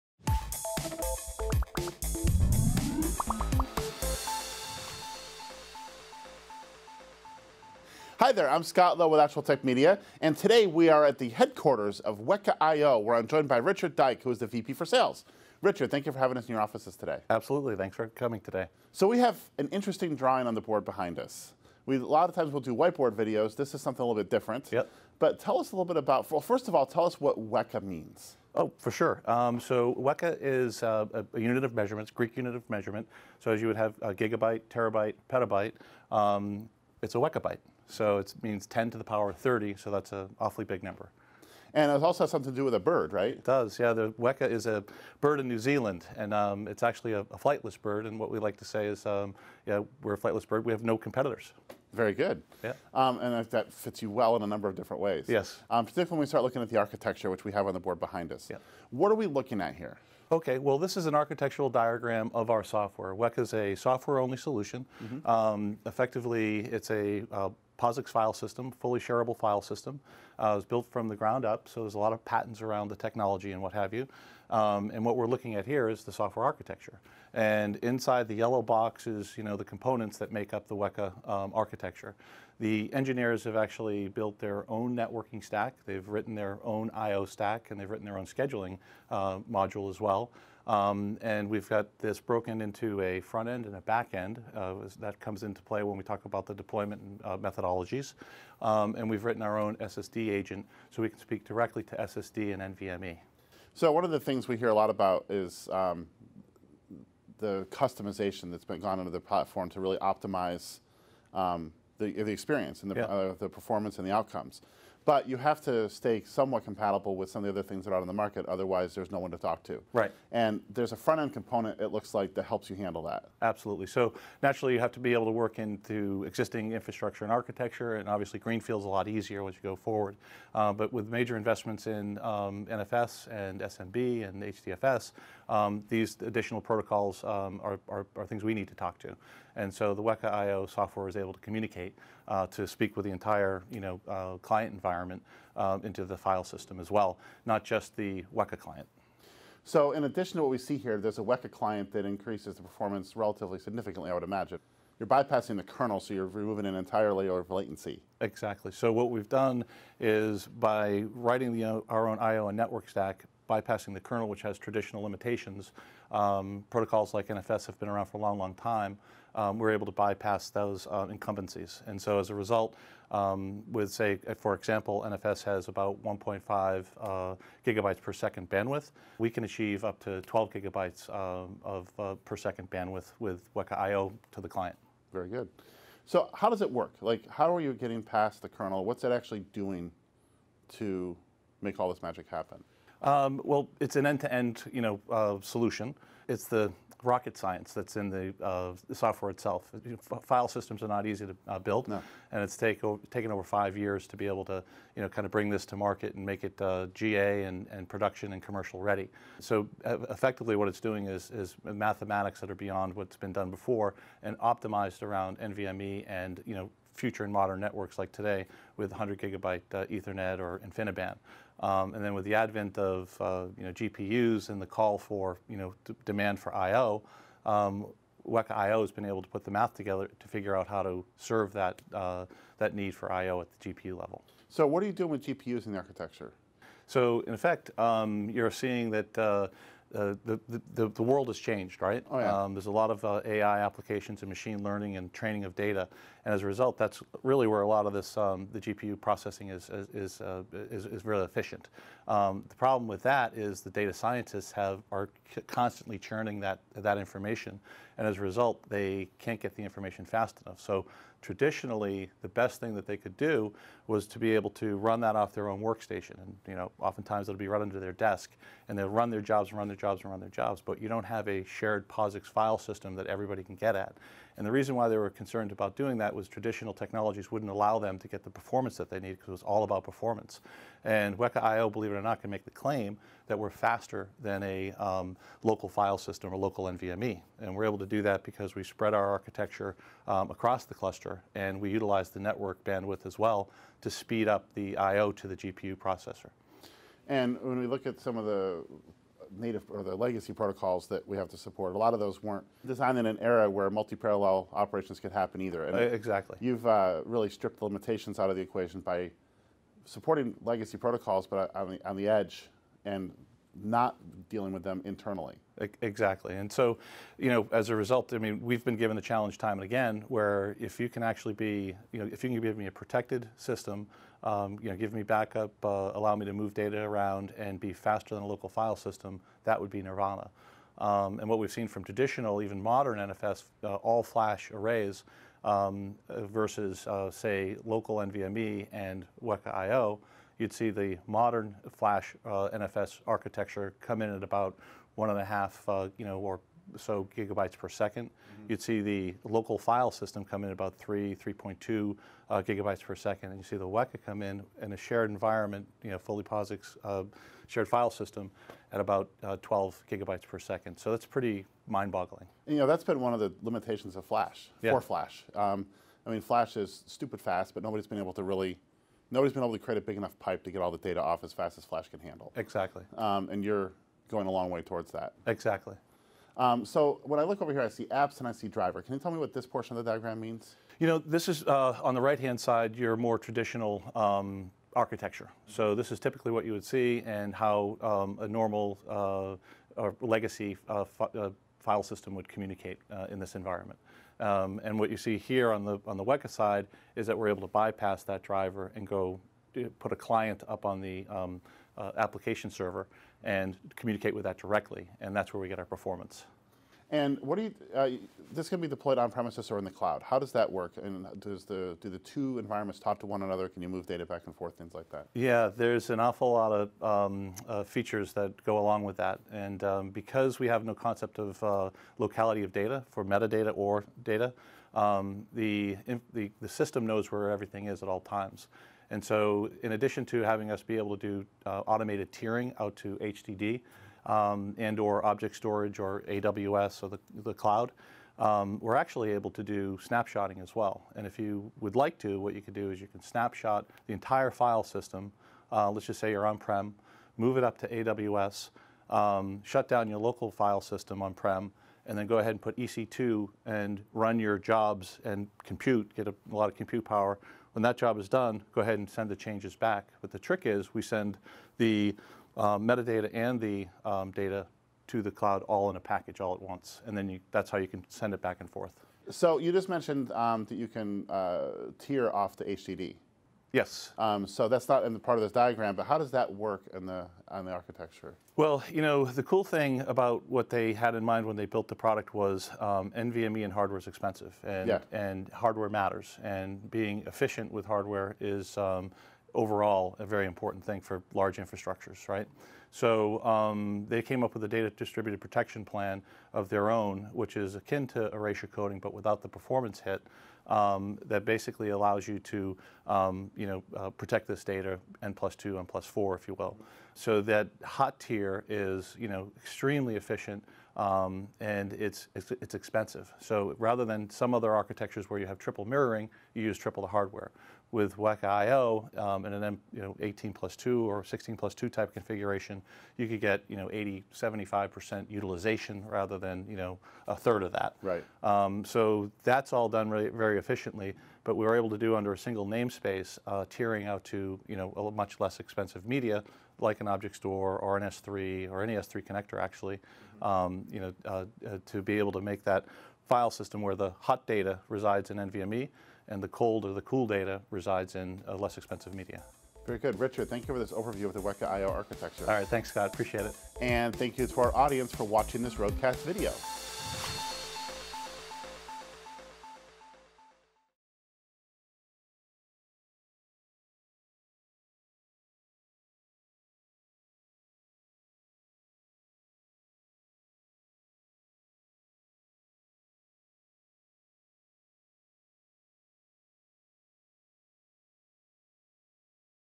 Could WekaIO Be The Fastest Filesystem on the Planet?
In June of 2018, the ActualTech Media team visited the offices of WekaIO to find out what they're doing in the world of filesystems. In this video, ActualTech Media CEO Scott D. Lowe interviews Richard Dyke, VP of Sales at WekaIO. In this interview, Richard talks to David about what sort of apps need a solution like WekaIO, what's different about WekaIO, and how it is quite possibly the fastest filesystem on the planet!